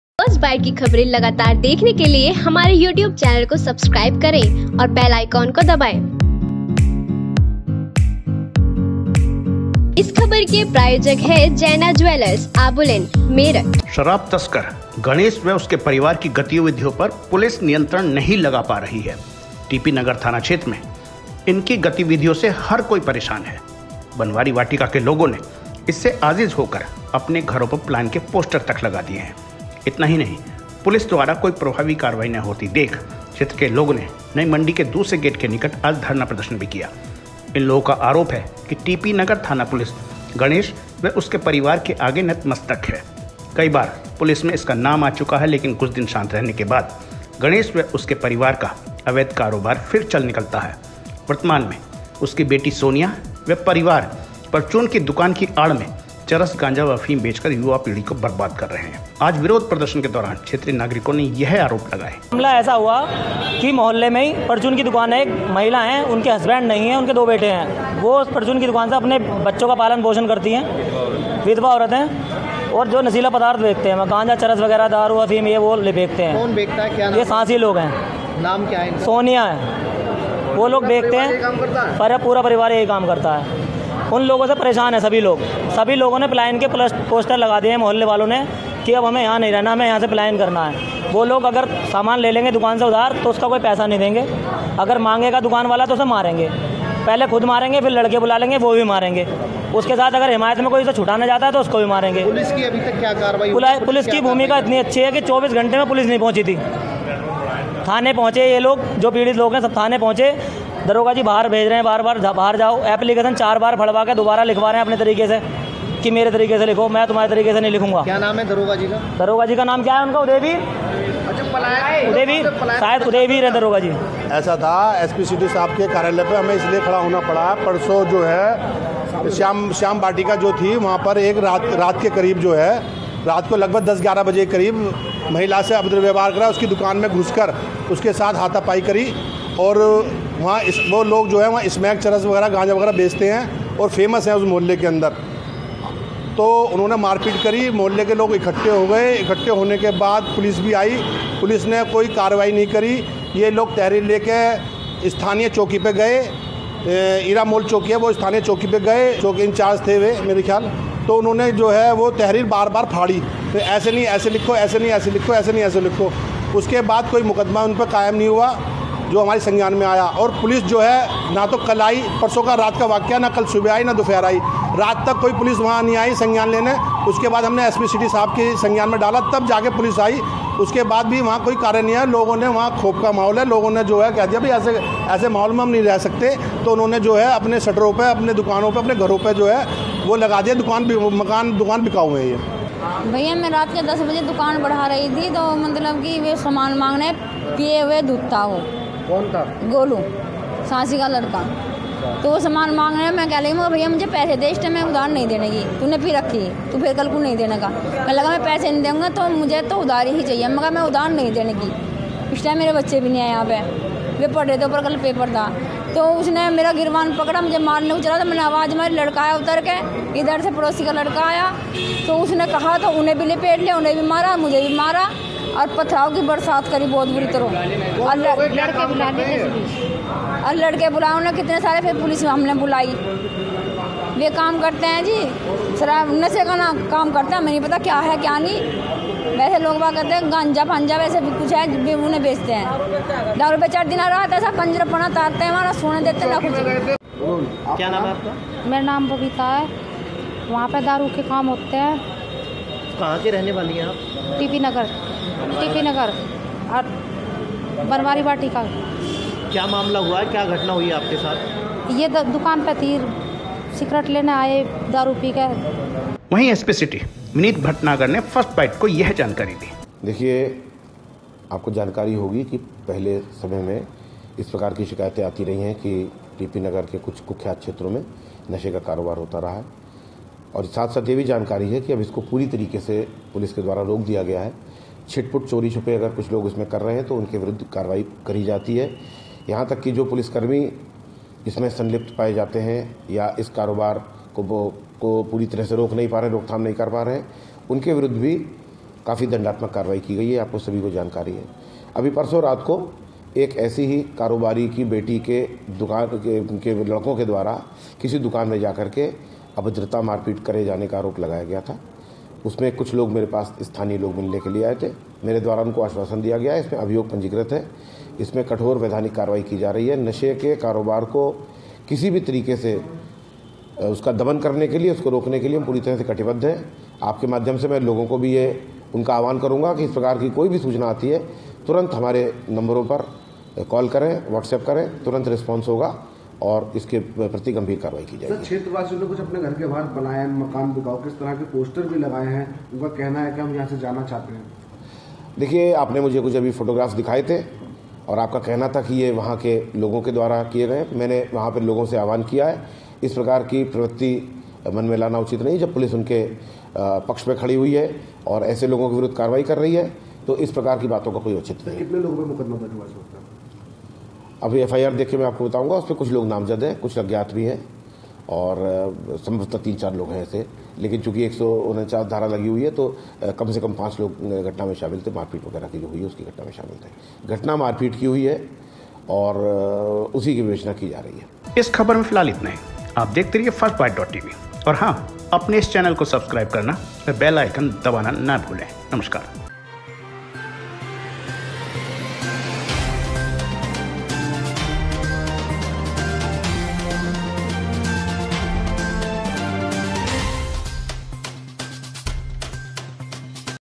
टीपीनगर में खुले आम मिलता है चरस गांजा, वाशिंदों ने किया विरोध प्रदर्शन । Firstbyte.Tv
शराब तस्कर गणेश की गतिविधियों पर पुलिस नियंत्रण नहीं लगा पा रही है। टीपीनगर थाना क्षेत्र में इसकी गतिविधियों से हर कोई परेशान है। बनवारी वाटिका के लोगों ने इससे आजिज होकर अपने घरों पर पलायन के पोस्टर चस्पा कर दिये। इतना ही नहीं पुलिस द्वारा कोई प्रभावी कार्यवाही न होते देख क्षेत्र लोगों ने नई मंडी के दूसरे गेट के निकट धरना दे दिया। इन लोगों का आरोप है कि टीपीनगर थाना पुलिस गणेश के आगे नतमस्तक है। कई बार पुलिस में इसका नाम आ चुका है लेकिन कुछ दिन शांत रहने के बाद गणेश का अवैध कारोबार फिर चल निकलता है। वर्तमान में उसके बेटी सोनिया व परिवार परचून की दुकान की आड़ में चरस गांजा अफीम बेच कर युवा पीढ़ी को बर्बाद कर रहे हैं ।